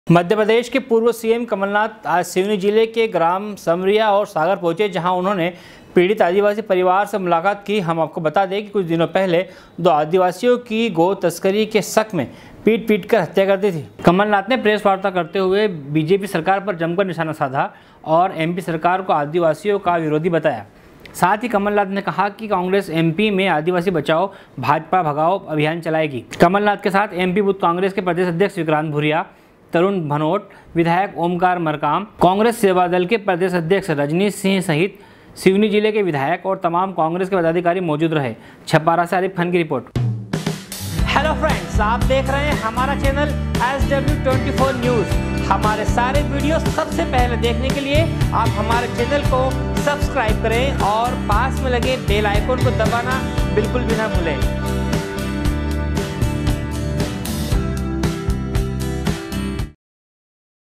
SW 24 News Madhyapradesh
मध्यप्रदेश के पूर्व सीएम कमलनाथ आज सिवनी जिले के ग्राम सिमरिया और सागर पहुंचे जहां उन्होंने पीड़ित आदिवासी परिवार से मुलाकात की हम आपको बता दें कि कुछ दिनों पहले दो आदिवासीयों की गोकसी के शक में पीट पीट कर हत्या कर दी गई थी...छपारा से आरिफ मोहम्मद की रिपोर्ट